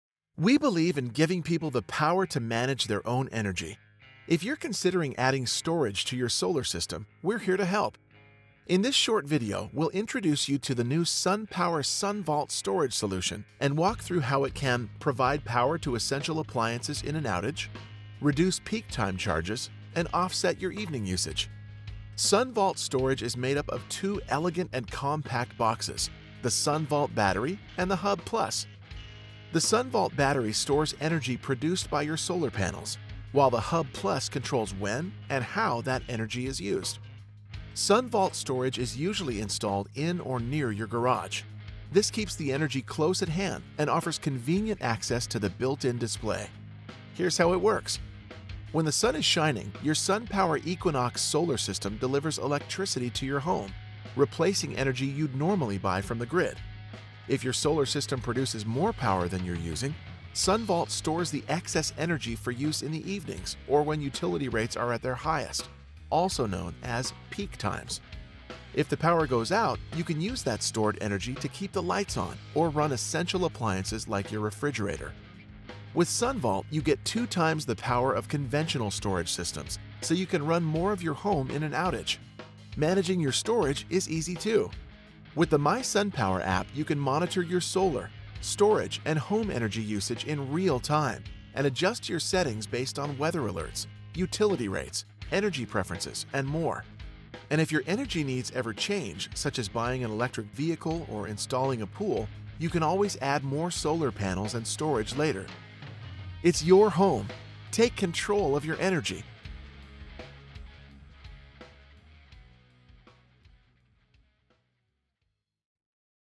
SunPower SunVault Solar Battery Storage System
Take a quick look at the components of SunVault™ storage and see why it’s a better way to get power when you need it most. SunVault™ Storage integrates with SunPower Equinox® solar systems, creating the only home solar + storage solution, designed and warranted by one company, giving you peace of mind.